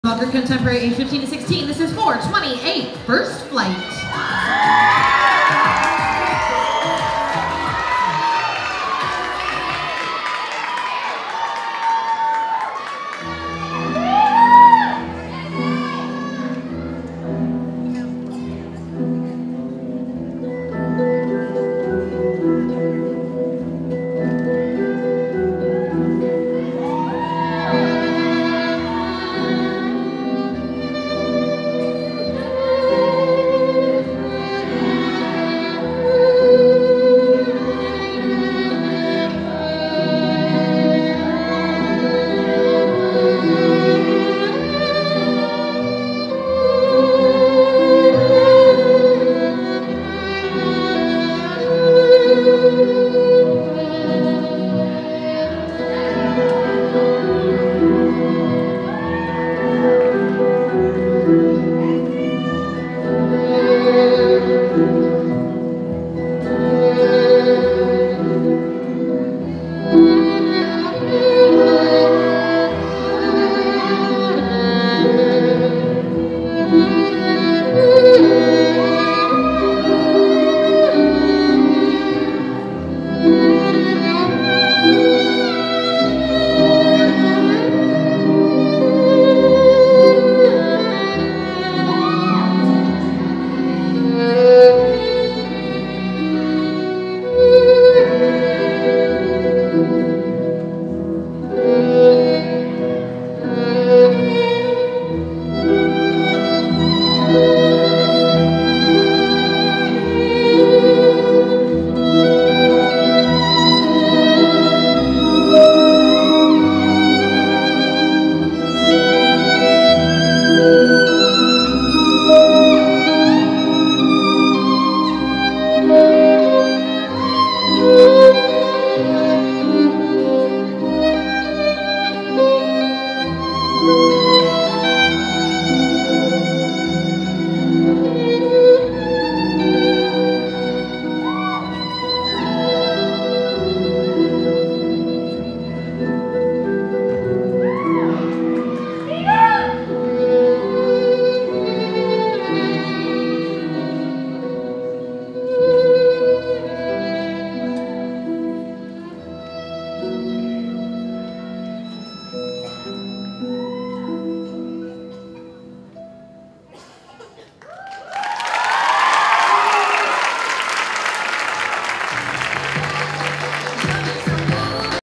Best Contemporary // FIRST FLIGHT - LINDA DOBBINS DANCE [Birmingham, AL]
2018 Industry Dance Awards - Outstanding Performance

Follow @industrydanceawards